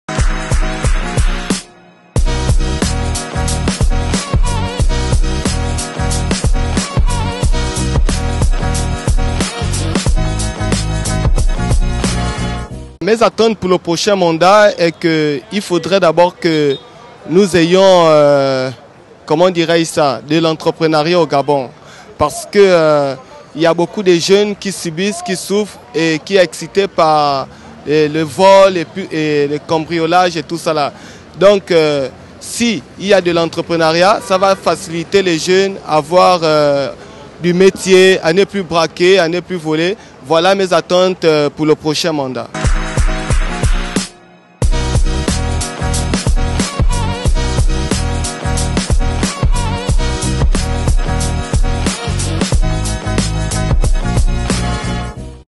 [Demain, C'est Nous] Épisode 1
En 2021, environ 60 % de la population du Gabon avait moins de 25 ans. Notre population est jeune, nos attentes sont nombreuses.

Parlez-nous des vôtres.